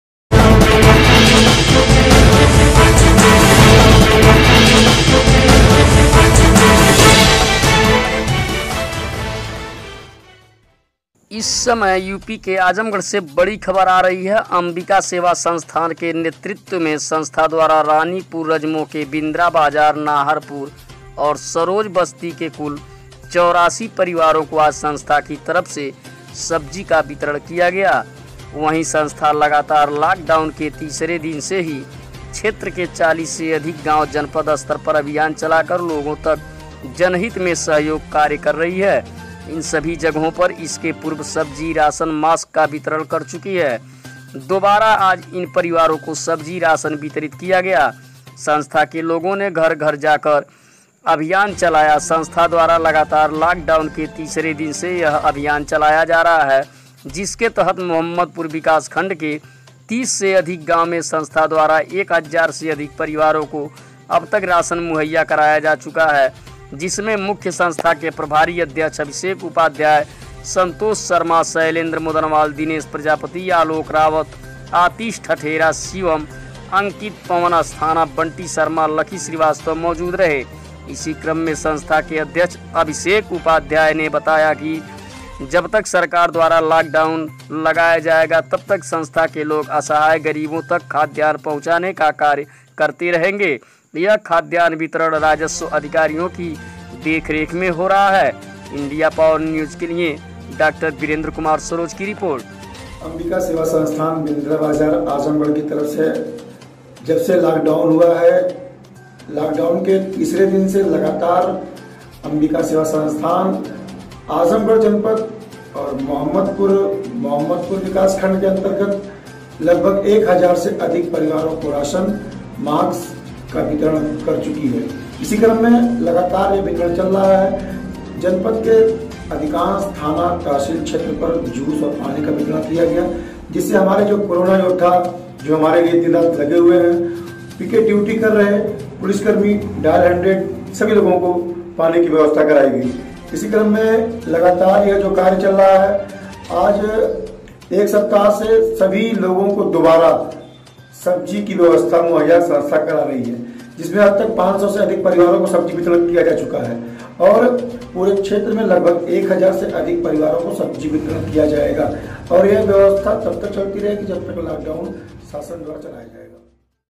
आज़मगढ़ Lock Down के तीसरे दिन से ही अम्बिका सेवा संस्थान द्वारा की जा रही मदद
आजमगढ़ ।अंबिका सेवा संस्थान  के नेतृत्व में संस्था द्वारा रानीपुर रजमो के बिंद्रा बाजार नाहरपुर और सरोज बस्ती के कुल 84 परिवारों को आज संस्था की तरफ से सब्जी का वितरण किया गया वहीं संस्था लगातार लाॅक डाउन के  तीसरे दिन से ही क्षेत्र के 40 से अधिक गांव जनपद स्तर पर अभियान चलाकर लोगों तक जनहित में सहयोग कार्य कर रही है  इन सभी जगहों पर इसके पूर्व सब्जी राशन मास्क का वितरण कर चुकी है दोबारा आज  इन परिवारों को सब्जी राशन वितरित किया गया संस्था के लोगों ने घर घर जाकर या अभियान चलाया संस्था द्वारा लगातार लाॅक डाउन के तीसरे दिन  से यह अभियान चलाया जा रहा है जिसके तहत मोहम्मदपुर विकासखंड के 30 से अधिक गांव में संस्था द्वारा 1000 से अधिक परिवारों को अब तक राशन मुहैया कराया  जा चुका है जिसमें मुख्य संस्था के प्रभारी अध्यक्ष अभिषेक उपाध्याय, संतोष शर्मा, शैलेंद्र मोदनवाल, दिनेश प्रजापति, आलोक रावत,  आतिश ठठेरा , शिवम ,अंकित, पवन अस्थाना  बंटी शर्मा  लकी श्रीवास्तव मौजूद रहे इसी क्रम में संस्था के अध्यक्ष अभिषेक उपाध्याय ने बताया कि जब तक सरकार द्वारा लॉक डाउन लगाया जाएगा तब तक संस्था के लोग असहाय गरीबों तक खाद्यान्न पहुंचाने का कार्य करते रहेंगे यह खाद्यान्न वितरण राजस्व अधिकारियों की देखरेख में हो रहा है।